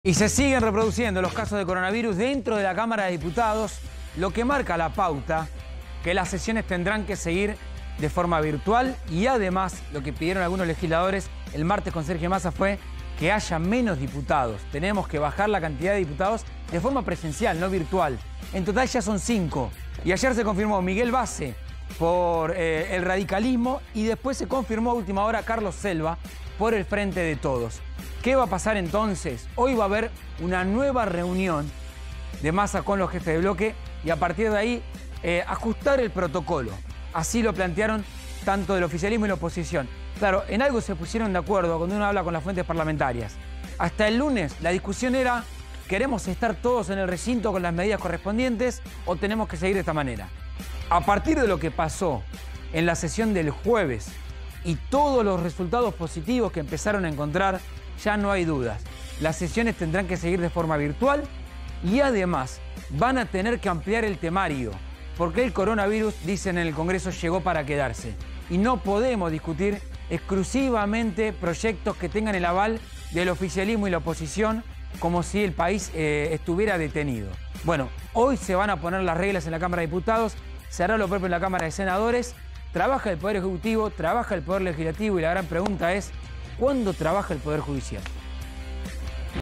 Otro diputado con coronavirus: Carlos Selva del Frente de Todos
Otro caso de Covid-19 : El diputado Carlos Selva, del Frente de Todos estuvo en la última sesión presencial.